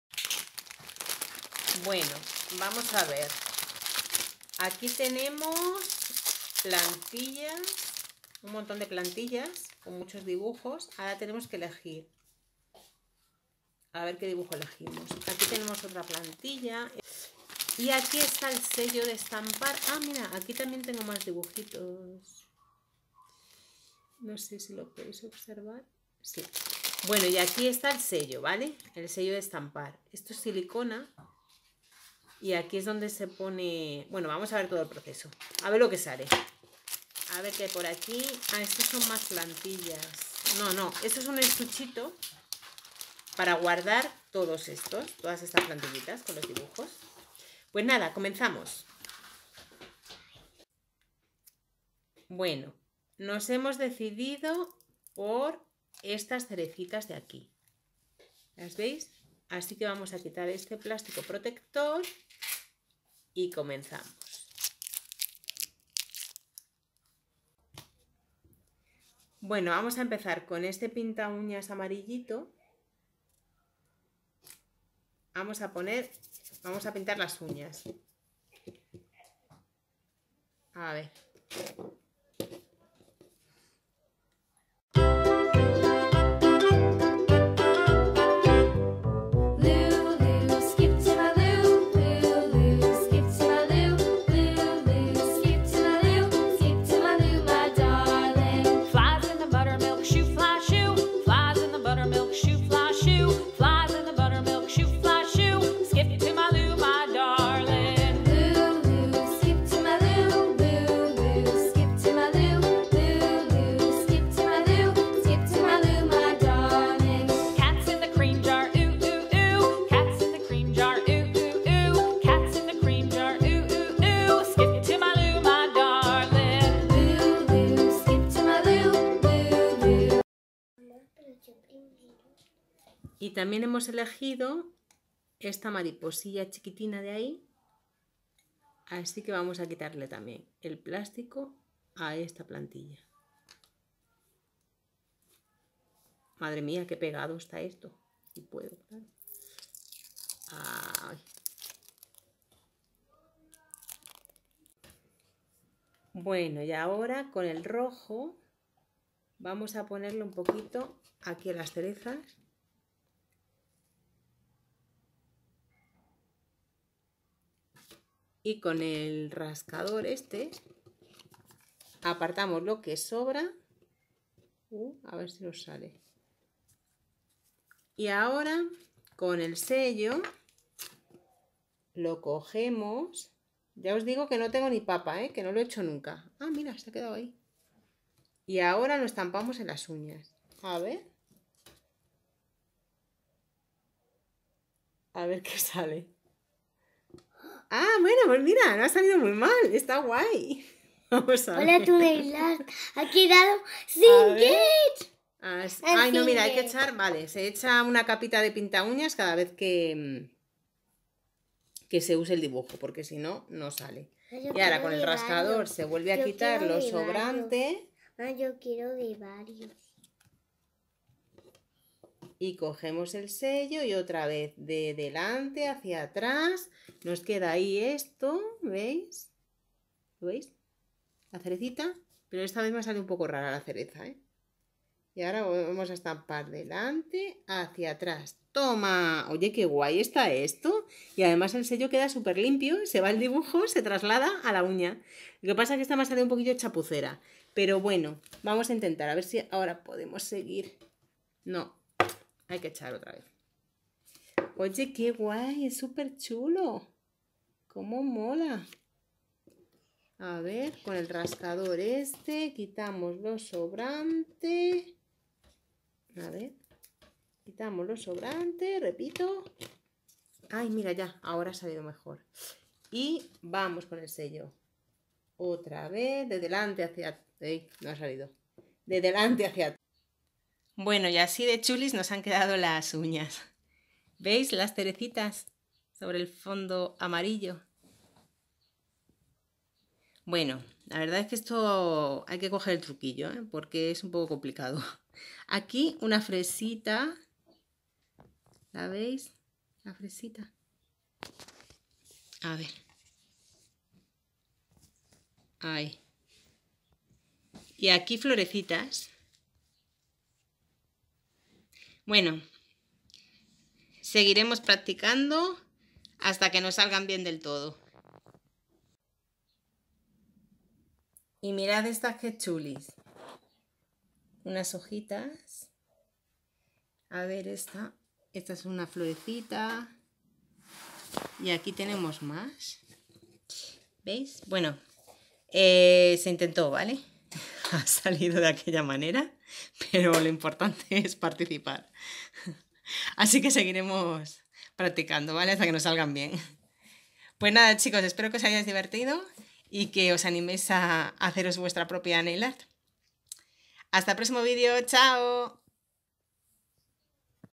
nos decoramos las uñas!!! NAIL ART
Nos decoramos las uñas con dibujos chulos.
Practicando... jeje